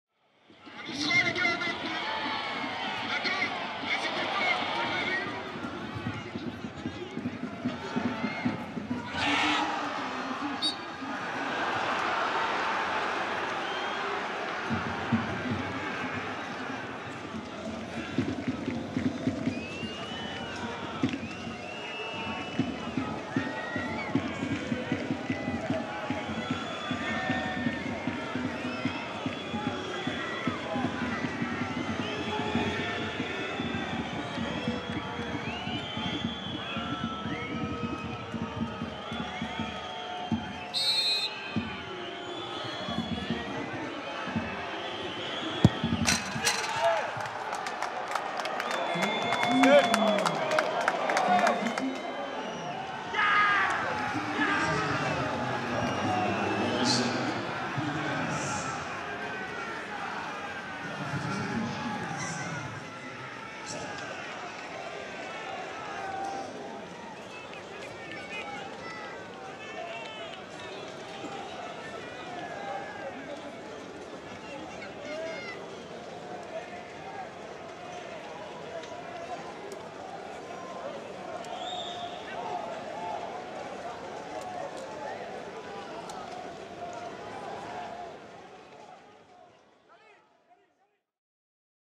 👟💥 Saison 23/24 - J17 | Le but de Ludovic Blas contre Clermont
90e+7 - Ludovic Blas, 3e buteur - L'attaquant rennais transforme en force le penalty obtenu quelques secondes auparavant par Bertug Yildirim. Ça fait 3-1 !